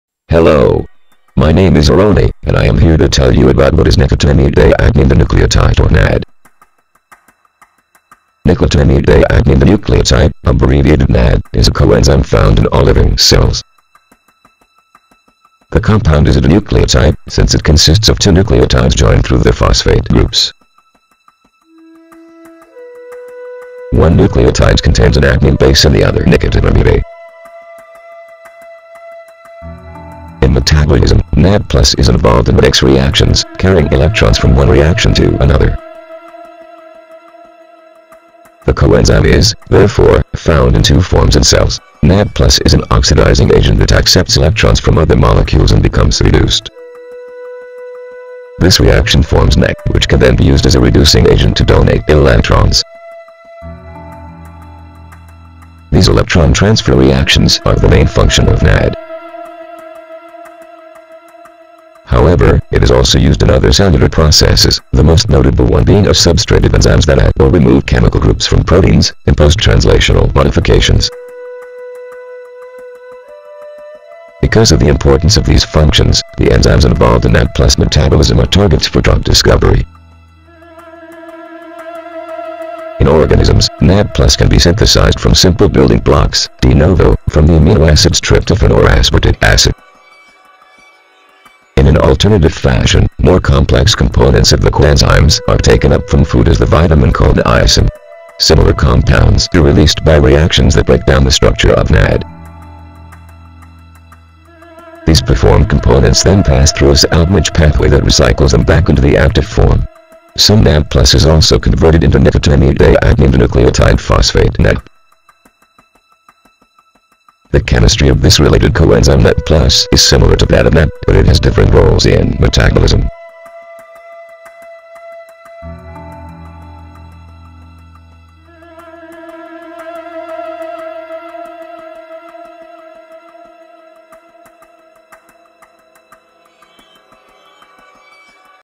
AP Biology - Nicotinamide Adenine Dinucleotide or NAD+
Get the App!

Nicotinamide adenine dinucleotide, abbreviated NAD+, is a coenzyme found in all living cells.